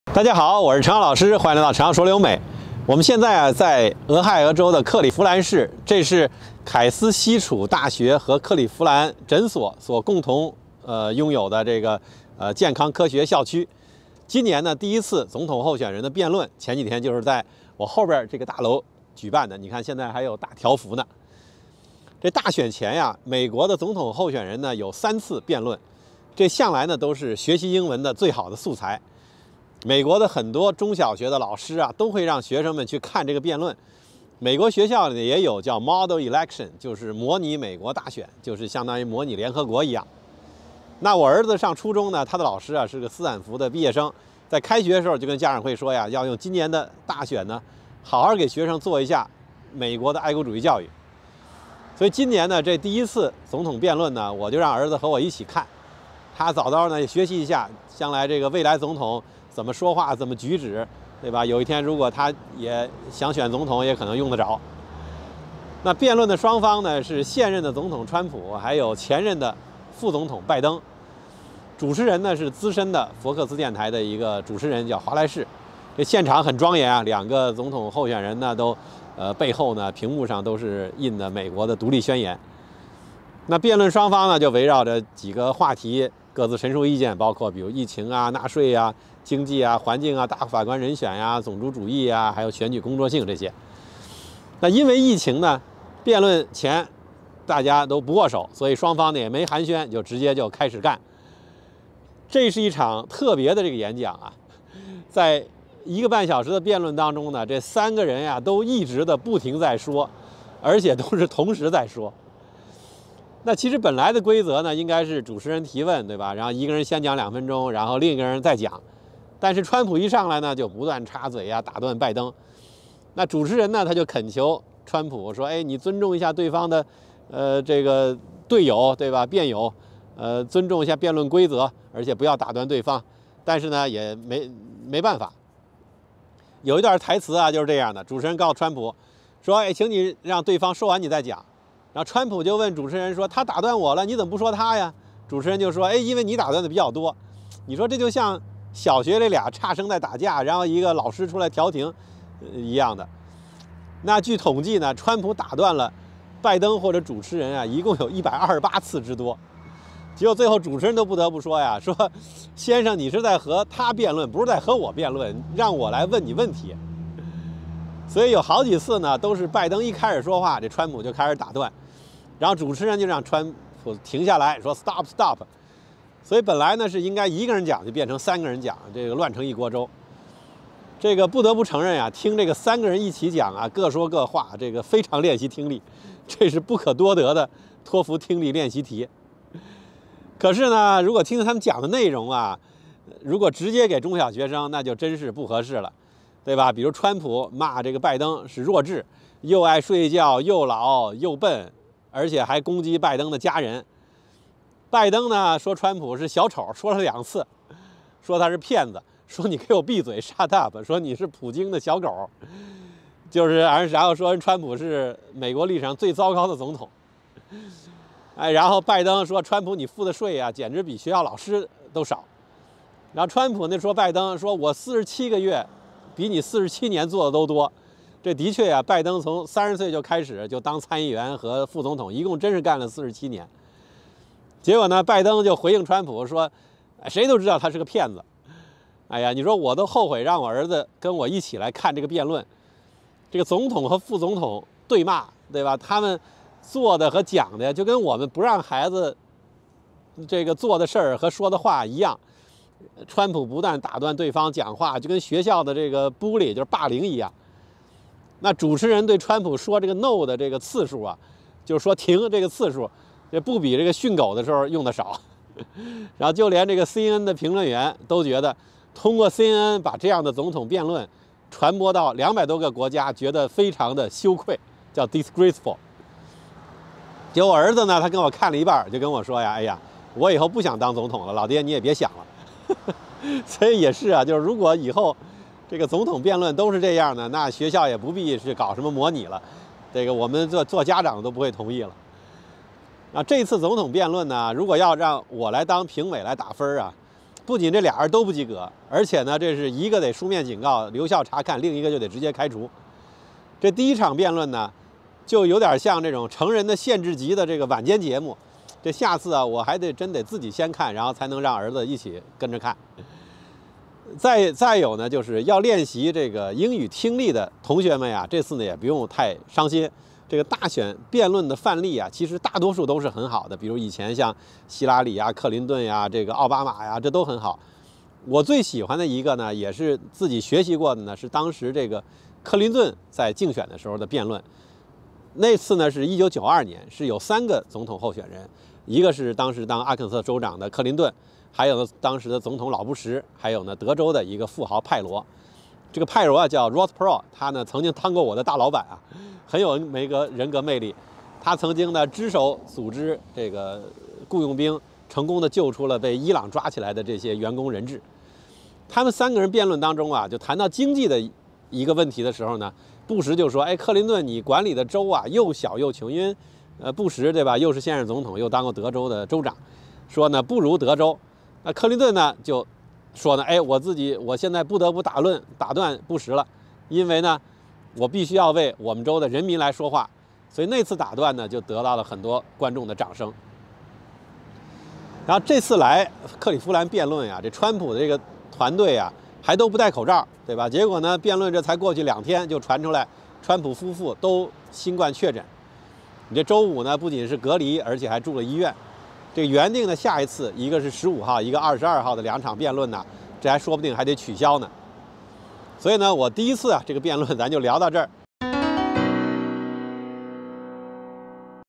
第183期 千万别把这次总统辩论当英语教材！失望透顶！
总统辩论向来都是学习英文的好素材，但是这次辩论令人失望透顶，言行举止都非常不妥，千万不要拿来作为英语素材！